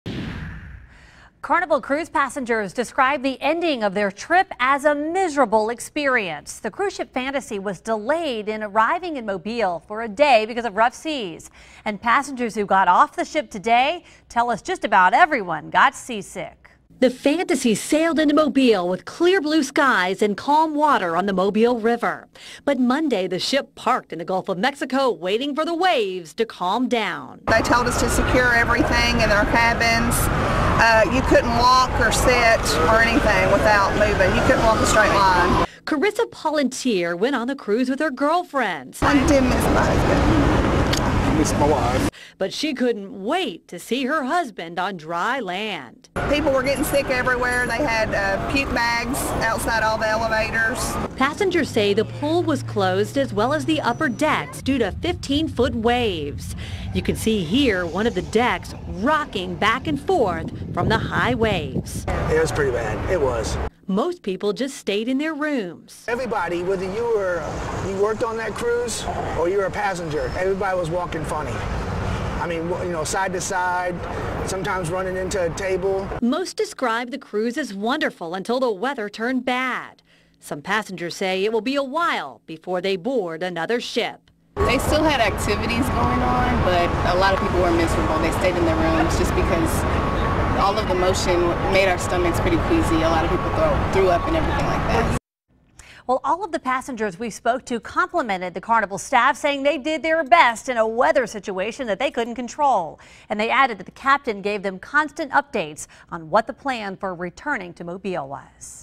WATCH: Cruise-goers Describe Trip as "Miserable" Due to Extreme Seas
Devon Walsh reports.